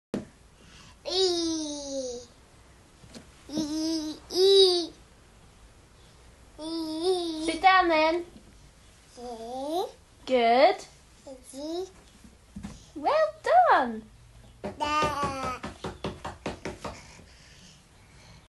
Huddy's playing on his caterpillar rocker
We got a really good deal on this rocker and Huddy absolutely loves it, thanks @argos